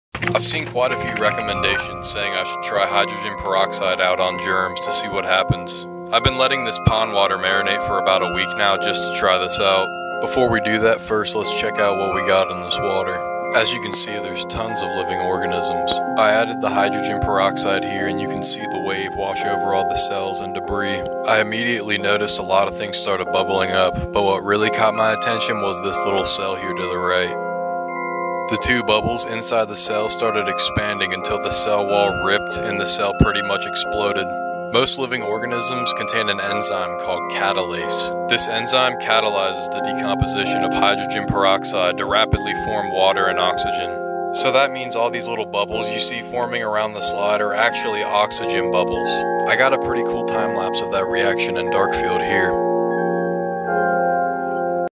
Watch H2O2 Kill Germs
H2O2 (One Hydrogen Atom Per Oxygen Atom) kills microbes on contact in two ways:

Being "unstable," it burns holes in cells and second it oxygenates/oxidizes cells.

Both are lethal to microbes.

This is ideal for internal bacteria, HOWEVER it also burns holes in human cells making it unhealthy even after being watered down to 3%. Low dosage simply means it burns holes in fewer cells. That's the only difference. That's why H2O2 turns skin cells white. The whiteness you see are the bursted cells. They are dead. It's very abrasive and dangerous and unhealthy to say the least.

Obviously it will also kill "healthy" gut bacteria (flora) too. But all internal bacteria gets sick, defecates, dies and rots inside the body so that adds to overall internal toxicity as well.

Due to UT's strict health rules, I cannot say what to do nor how to apply it properly without penalties and must just leave it here. Sorry in advance.